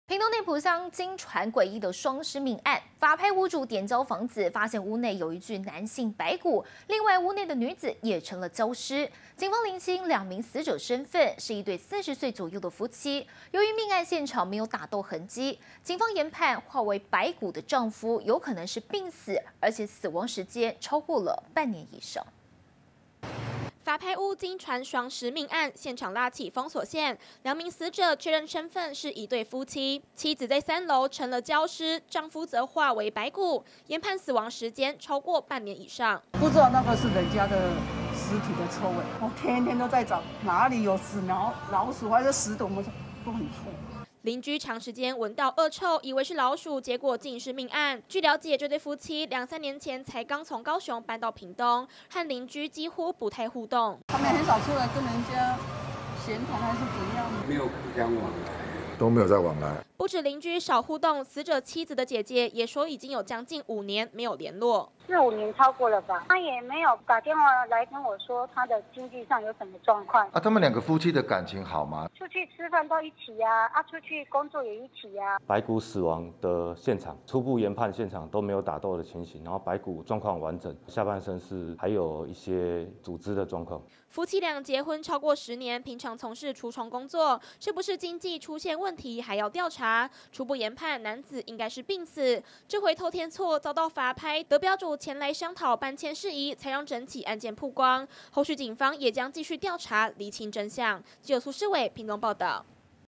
法拍屋離奇雙屍命案 白骨夫疑病死逾半年
屏東內埔鄉，驚傳詭異雙屍命案，法拍屋主點交房子，發現屋內有一具男性白骨，屋內女子也成了焦屍，警方釐清2名死者身分，是一對40歲左右的夫妻，由於命案現場，沒有打鬥痕跡，警方研判，化為白骨的丈夫，有可能是病死，死亡時間超過半年以上。